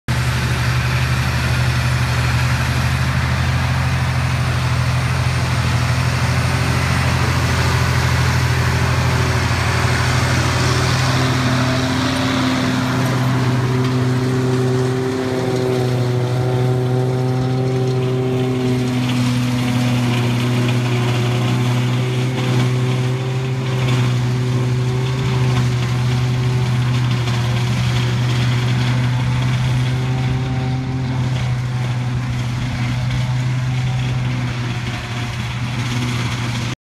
Dion F41 forage harvester and B58 rear unload forage box and trailer
First day in the field with a new Dion F41 harvester equipped with the "Stinger" spout.

Contact Jeff at Gibbsville Implement for more information or to receive a price and freight quote.